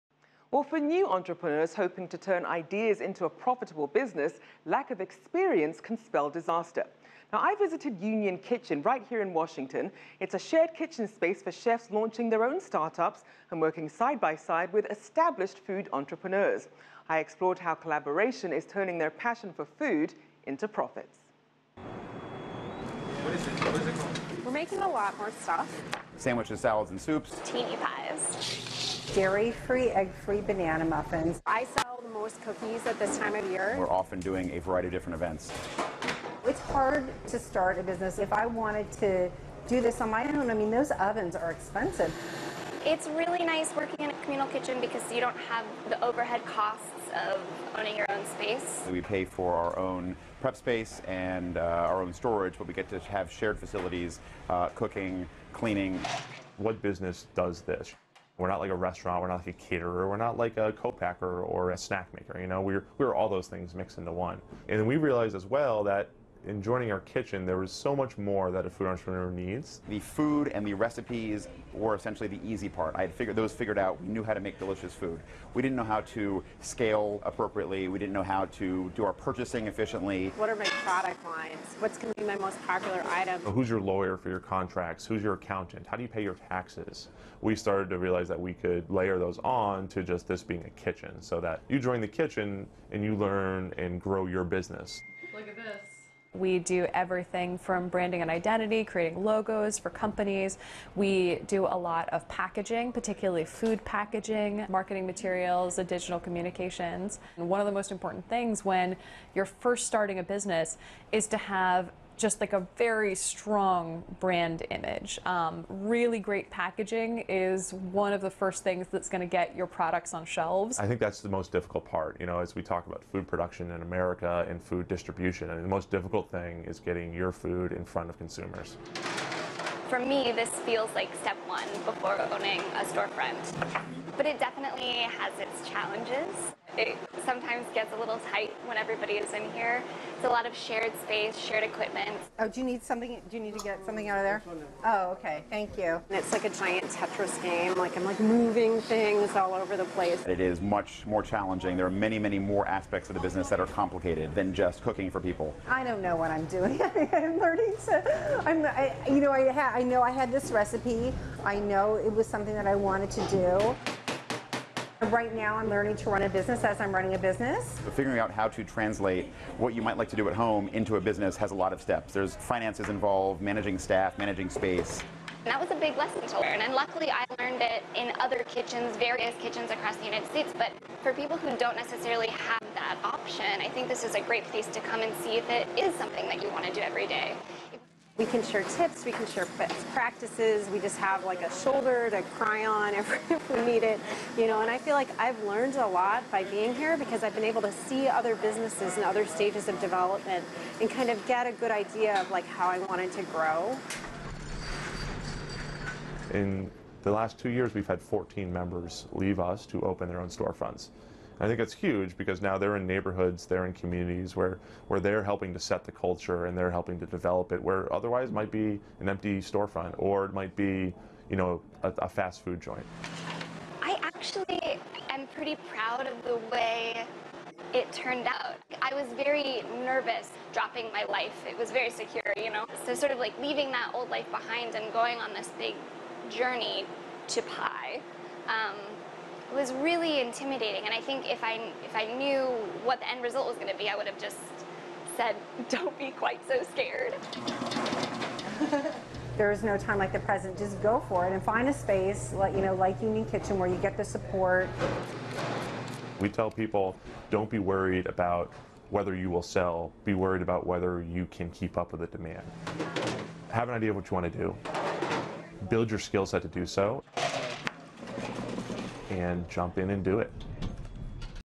DC's Union Kitchen helps food entrepenuers collaborate
Union Kitchen, a shared kitchen space in Washington D.C., allows new chefs to collaborate - and learn from seasoned professionals.